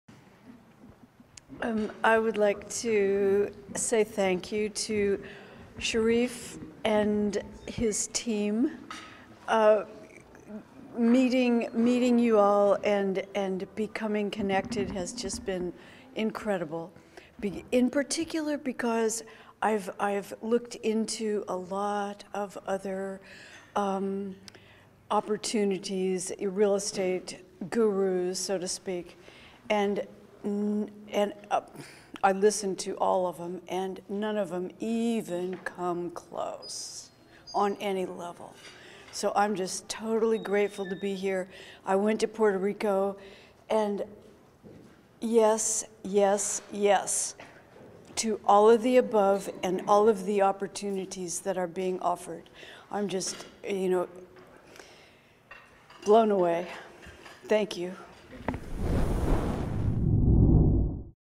There's None That Even Comes Close to Cherif!
Mary J,  shares her experience at a recent CMREI training event.